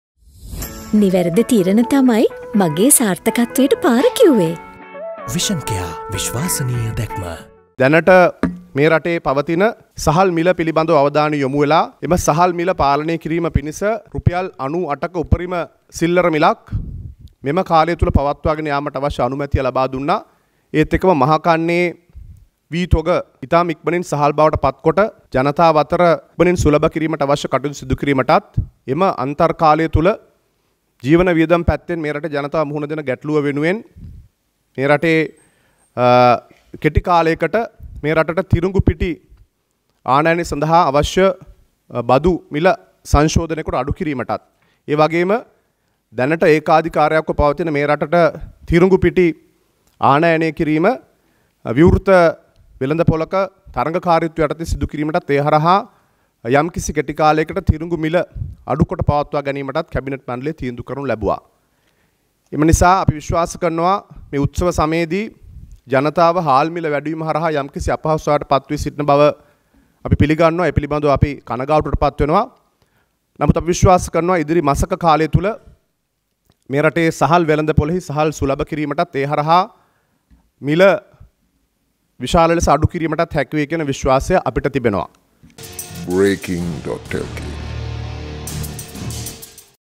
සහල් සඳහා උපරිම සිල්ලර මිළක් රජය තීරණය කරයි.| Breaking.lk
සහල් සඳහා උපරිම සිල්ලර මිළක් රජය තීරණය කරයි.